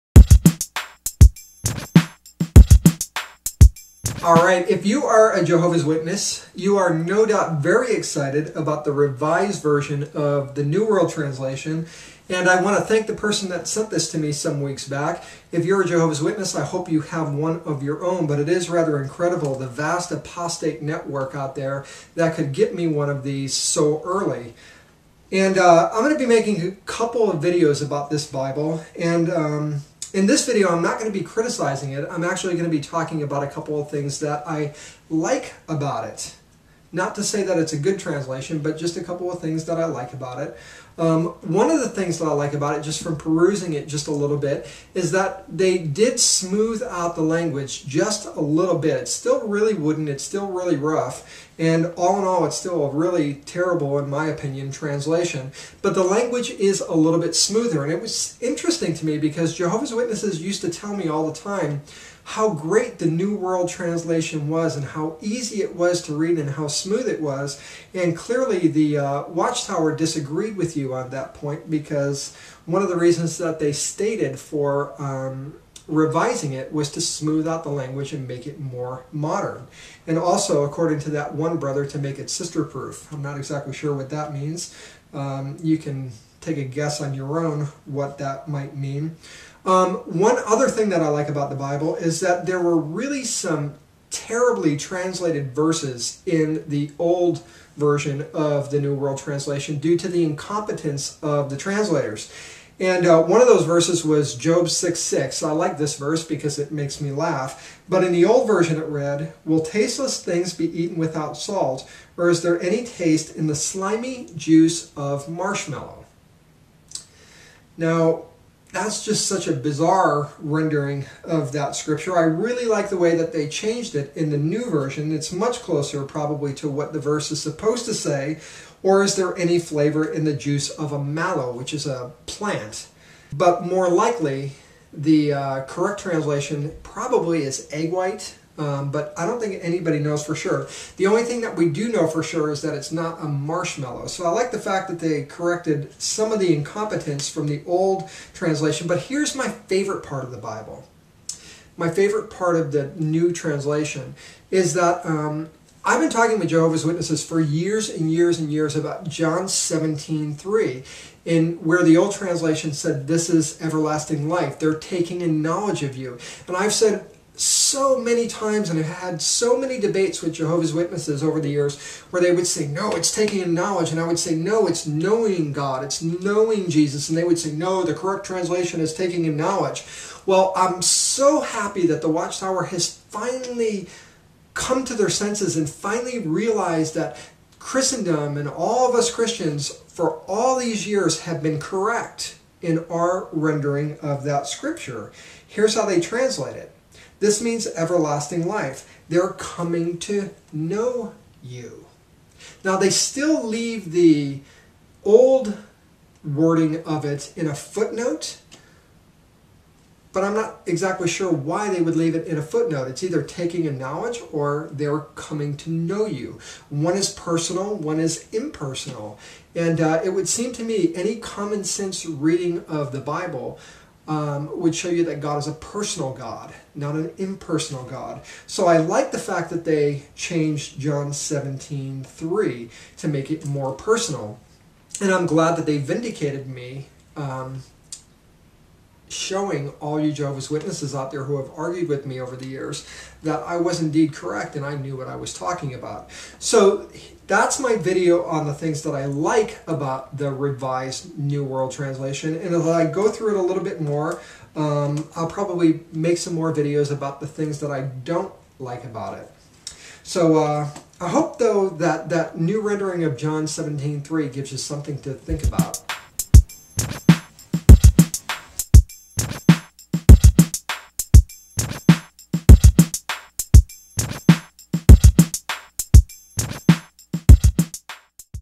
What I Like About The Revised New World Translation Jehovah's Witnesses Issue
What I Like About The Revised New World Translation and why a bunch of Jehovah's Witnesses  owe me an apology.